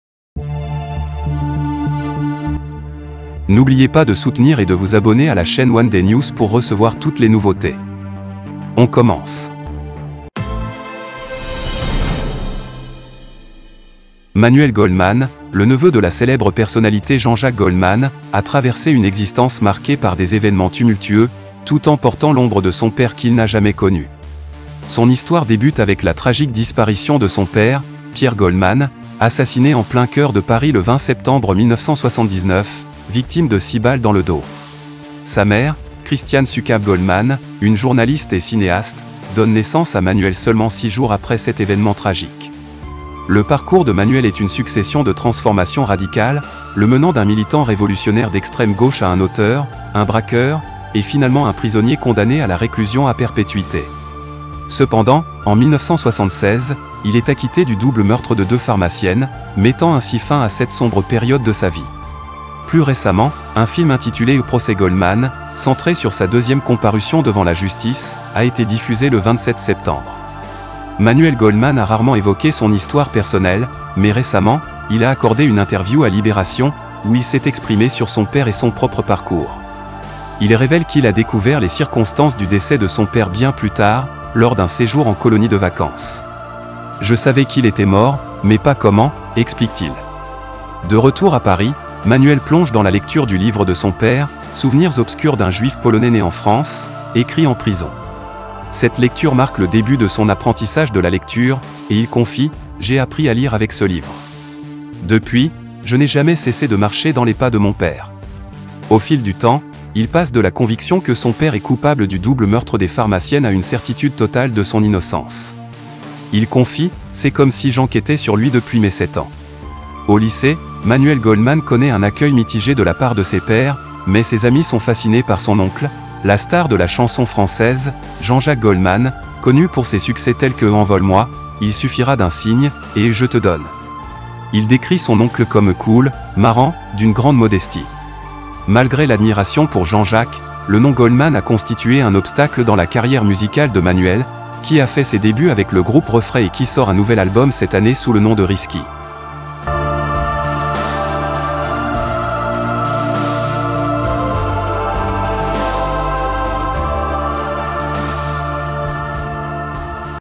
Le Lourd Héritage de Jean-Jacques Goldman : Son neveu Manuel sort du silence !
De retour à Paris, Manuel plonge dans la lecture du livre de son père, "Souvenirs obscurs d'un juif polonais né en France", écrit en prison. Cette lecture marque le début de son apprentissage de la lecture, et il confie : "J'ai appris à lire avec ce livre. Depuis, je n'ai jamais cessé de marcher dans les pas de mon père." Au fil du temps, il passe de la conviction que son père est coupable du double meurtre des pharmaciennes à une certitude totale de son innocence. Il confie : "C'est comme si j'enquêtais sur lui depuis mes 7 ans."